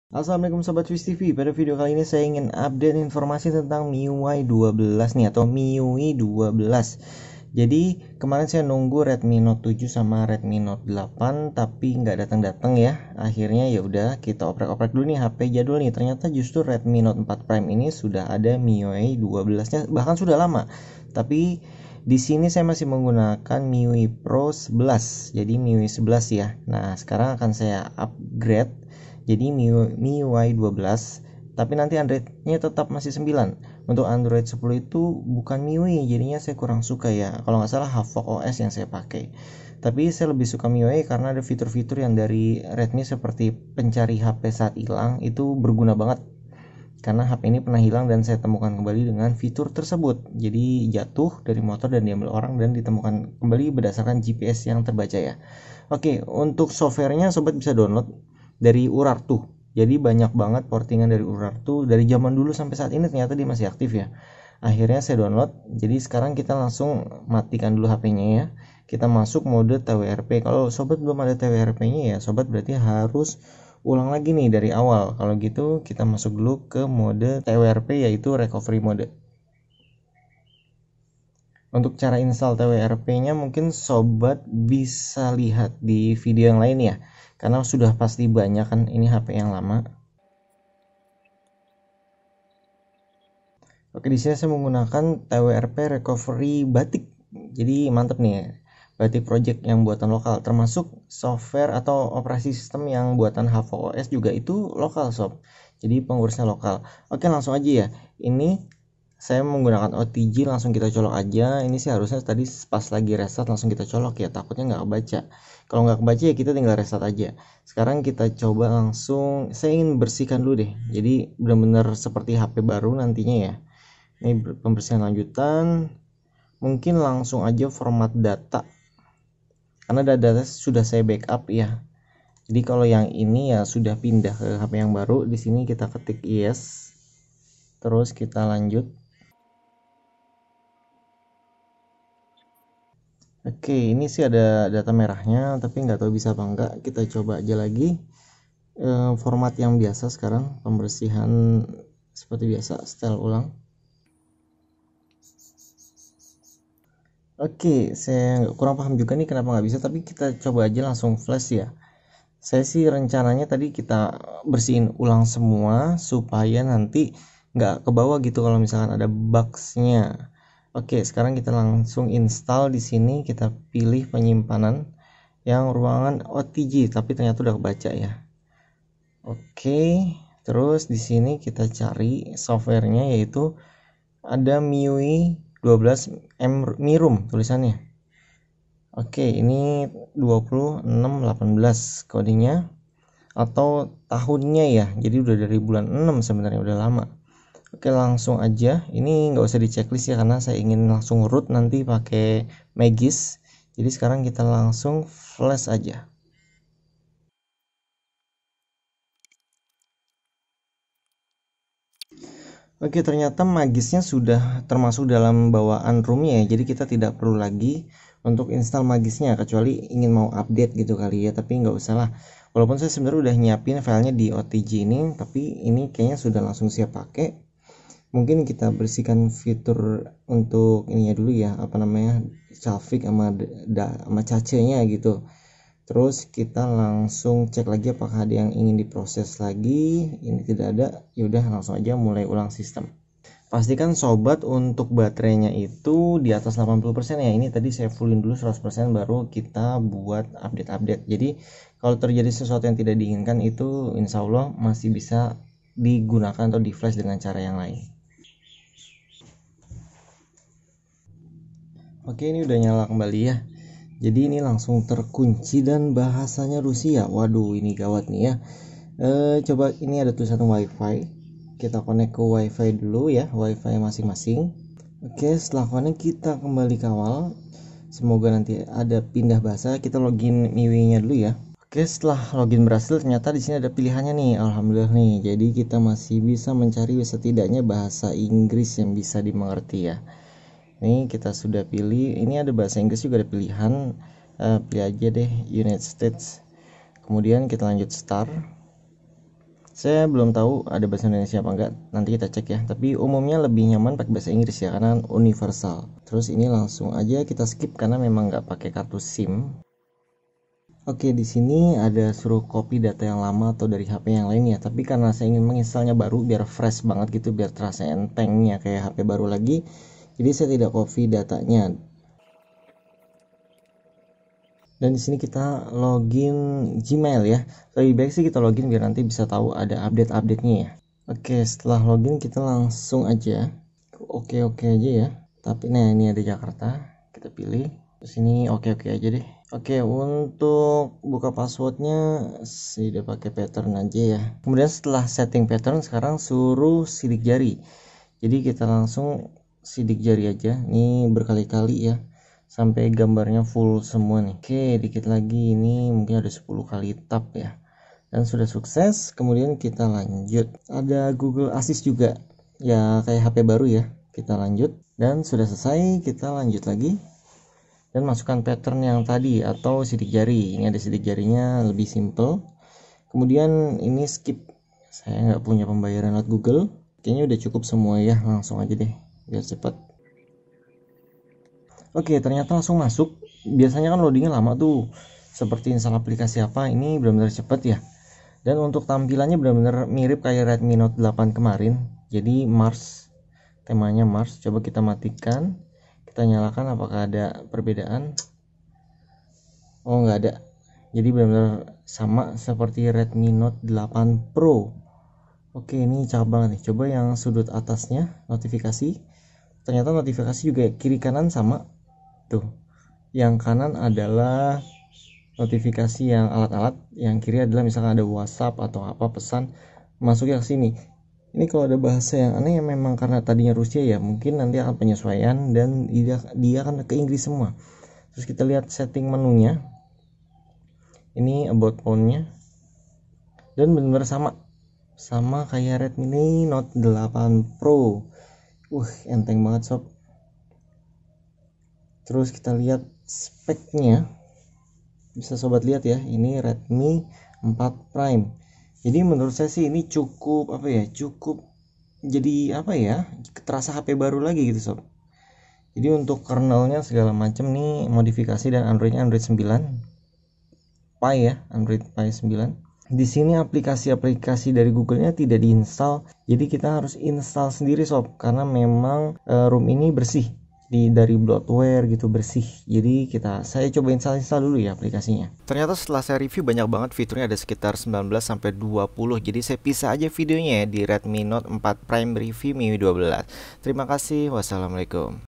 Cara Upgrade MIUI 12 Redmi 4 Prime Markw - Berasa HP baru lagi
Cara Upgrade MIUI 12 Redmi 4 Prime Markw - Jadi HP Baru lagi seperti Redmi 9C. Walaupun HP Redmi 4 Prime datang dari masa lampau, tapi jika masih mendapatkan update terbaru. Maka rasanya akan seperti hp zaman now yang banyak fitur Modern kekinian.